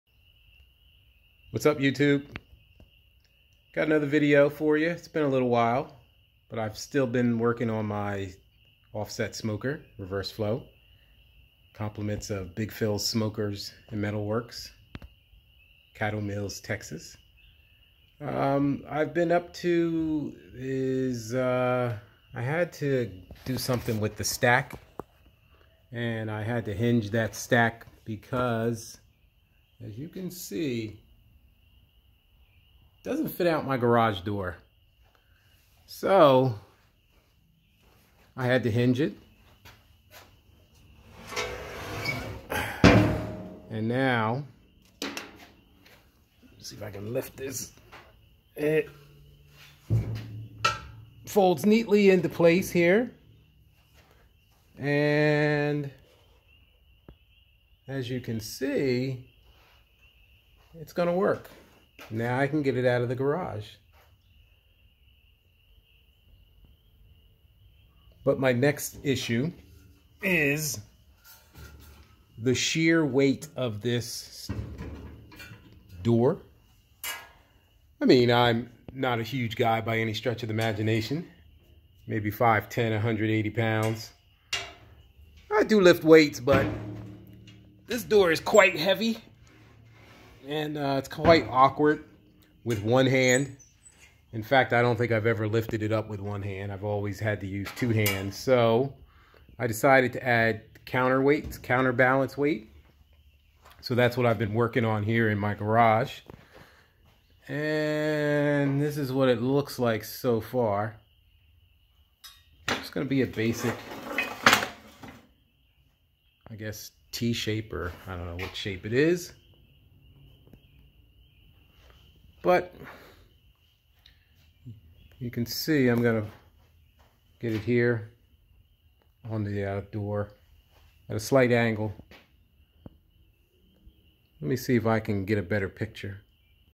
Reverse Flow offset smoker build update.
Reverse Flow offset smoker. just an update on what's been happening with my build.